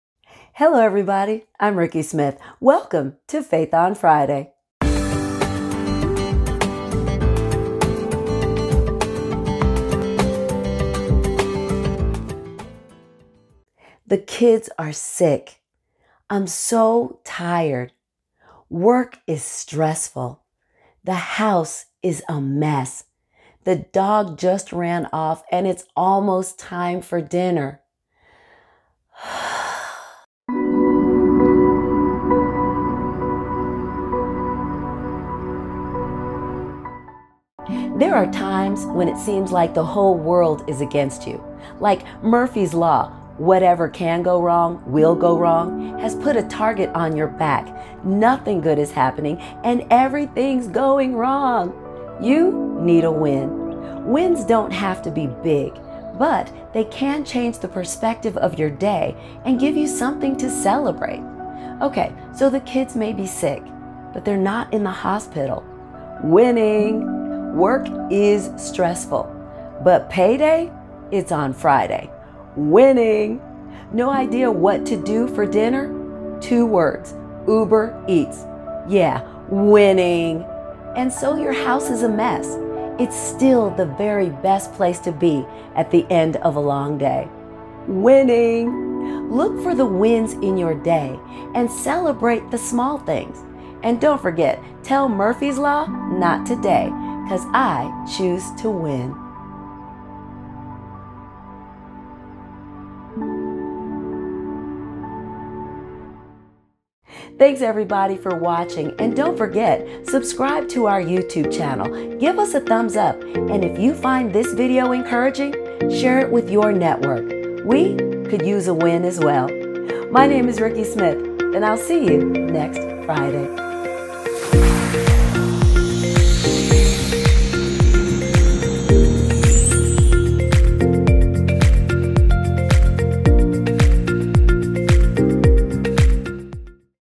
Faith on Friday - Take the win
When it seems that everything is going wrong and noting will go right, you need a win. Winning is about looking for the positives in a bad/wrong situation. We can all use a win.